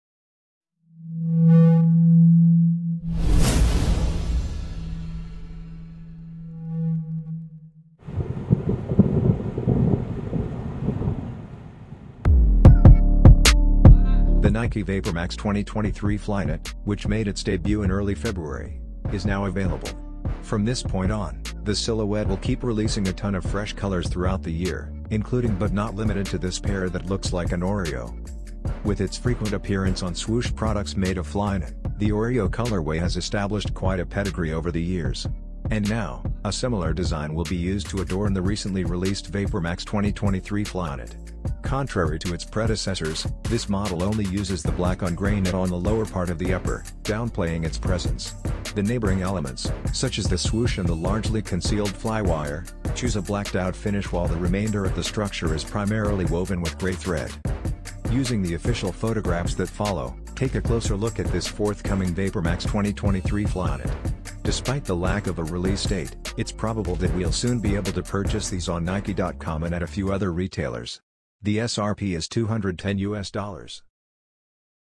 OREO VIBES 2023 Nike Air Vapormax Flyknit 2023 DETAILED LOOK + PRICE
Nike Air VaporMax 2023 Flyknit
OREO
2023

FOR ADULTS
LIFESTYLE

Style Code: DV1678-004
Release Date: SUMMER 2023
Price: US$210